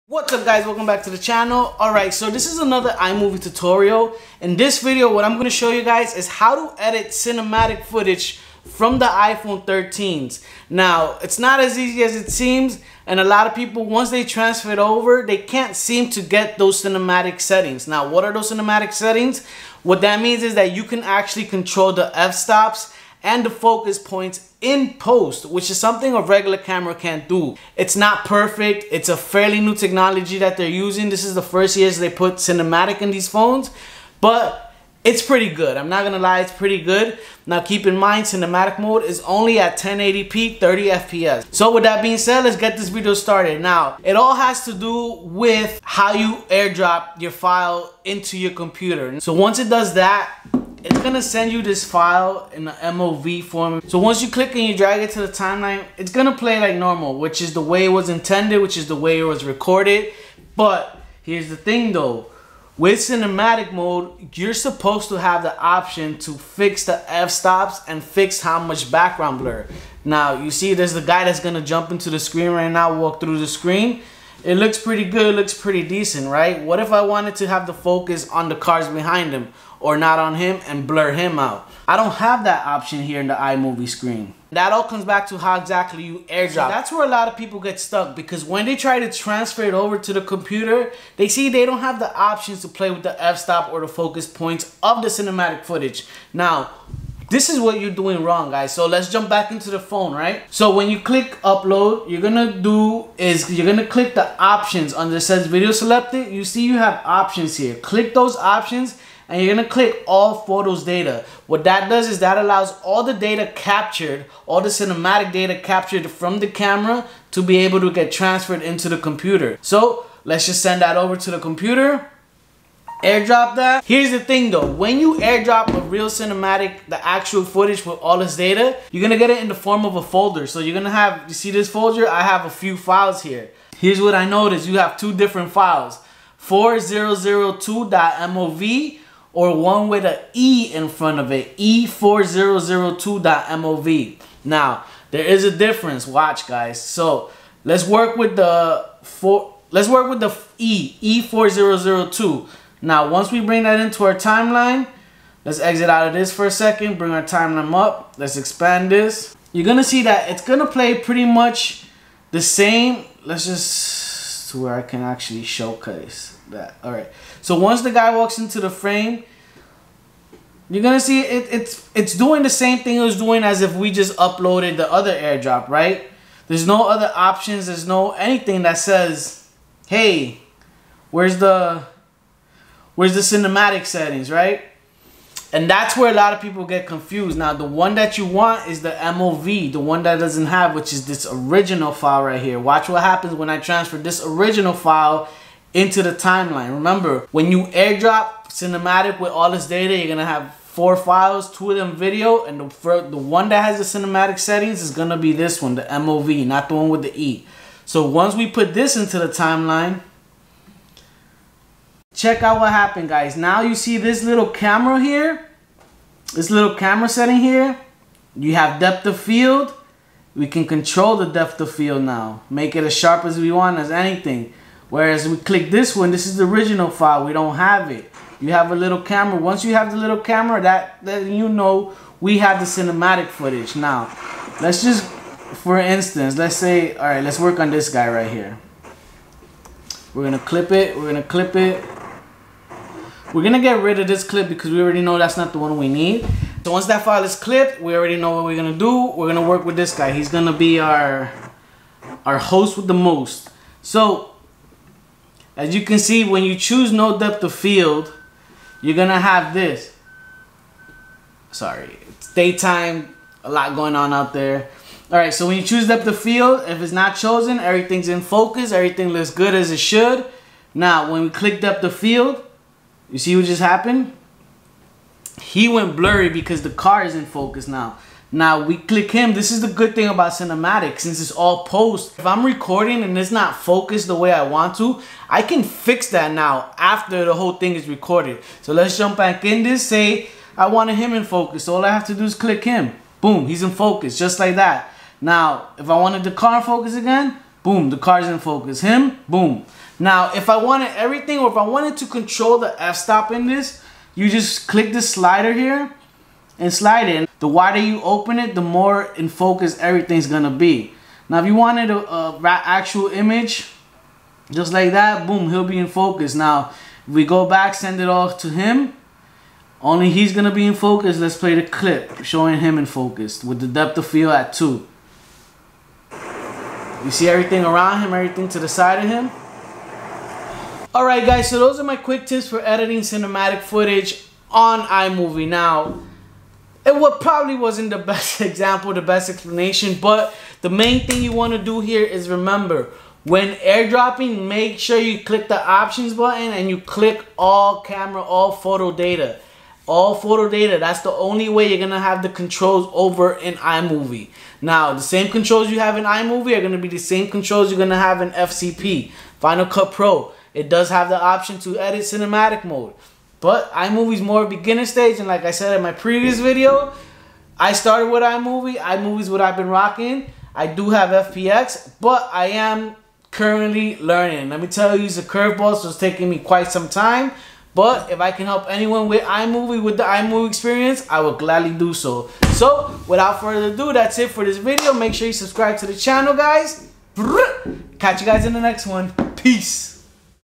Edit CINEMATIC iPhone 13 footage in iMovie
The iPhone 13 lineup has a cinematic feature in the camera, but how do we edit this footage in iMovie. In this tutorial I’m showing you the correct way to edit and upload cinematic footage in iMovie. It’s not as easy as air dropping and go. There are a few steps to get this done. Hope you find this video helpful.